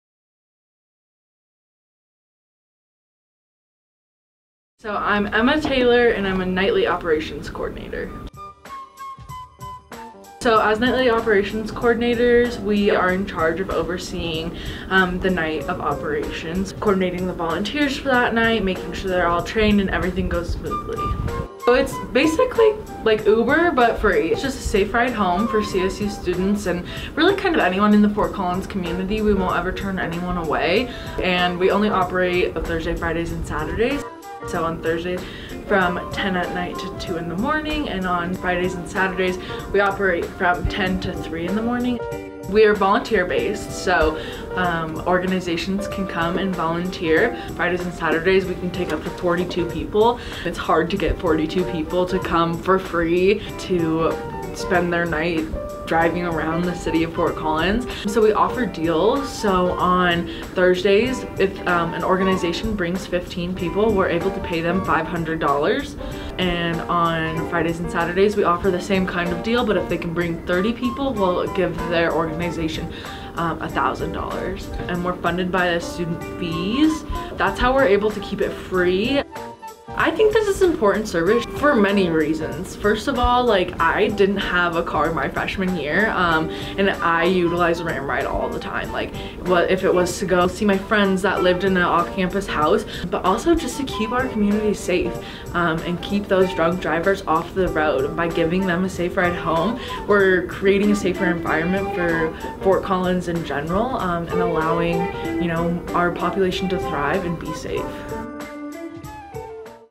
#30 Volunteer For Ram Ride | 70 Things To Do @ CSU 360°
In this series we will be going through the list of 70 thing every Ram is tasked with completing during their time here at Colorado State University. In this episode we explore Ram Ride, an amazing and unique service offered at CSU! Join us on this immersive journey through our school and witness a portion of the amazing things it has to offer.